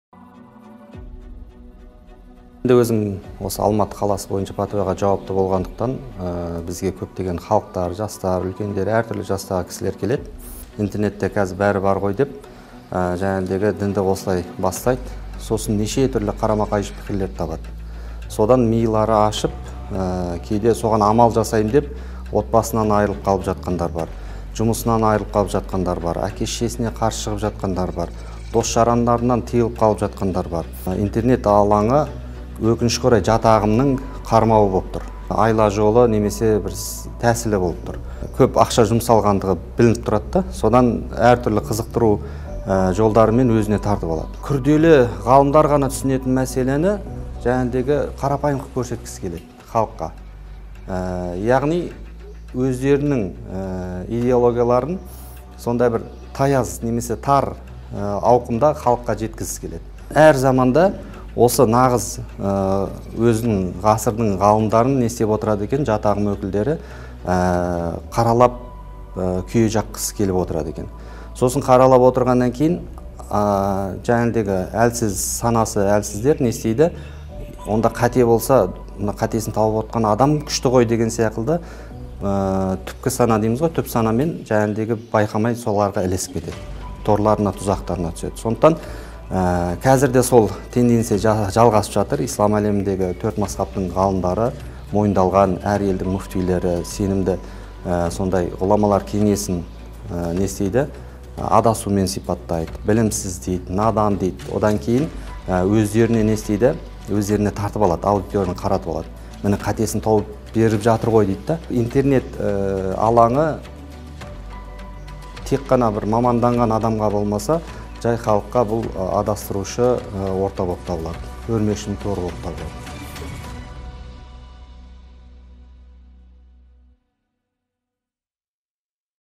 Сауаты жоққа - сайтан ұстаз
Деструктивті діни топтардың интернетті  қару еткендеріне 10 жылдан асты. Бұл аралықта әлемнің түкпір-түкпірінен псевдоуағызшылардың арандатуына түсіп қалғандардың көбі қарулы қақтығыс аймақтарынан бірақ шықты.
Жастарды нысанға алған экстремистік топтардың мақсаты – мемлекет тұтастығына сызат түсіріп, ел ішінде алауыздық  тудырып, шаңырақты шайқалту.
Мамандар: «сақтанудың жалғыз жолы – ақпаратты сана сүзгісінен өткізіп, сенімді сайттарға  жүгінген абзал»  дейді.